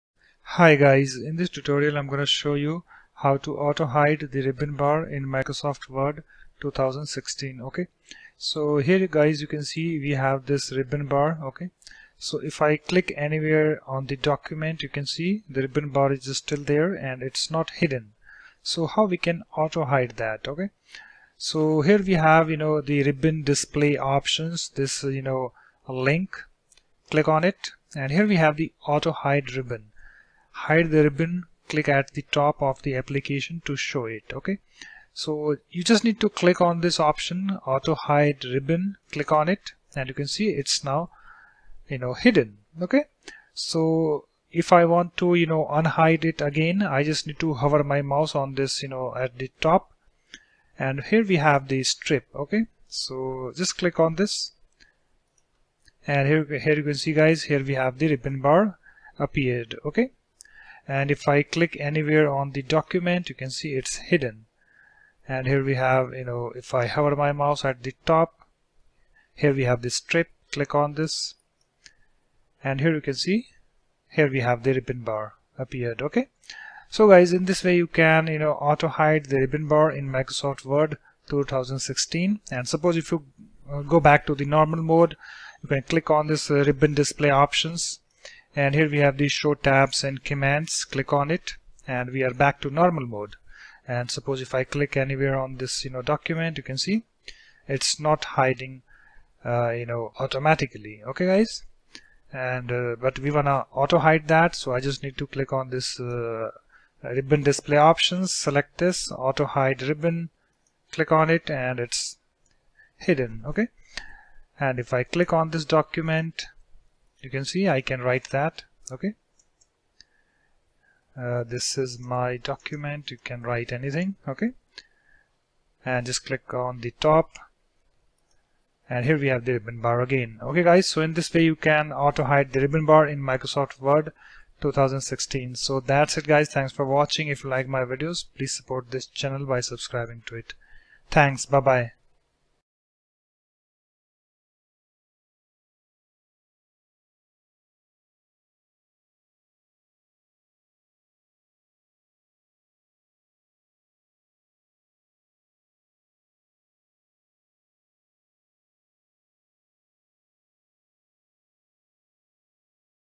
How to Auto-Hide the Ribbon Bar in Microsoft Word 2016?
Step 1: Open Ms Word, and then click on 'Ribbon Display Options' next to Sign-in button.

Step 2: Click on 'Auto-Hide Ribbon.'

If you want to display ribbon bar again, hover your mouse at the top in Ms Word, click on strip.

It's done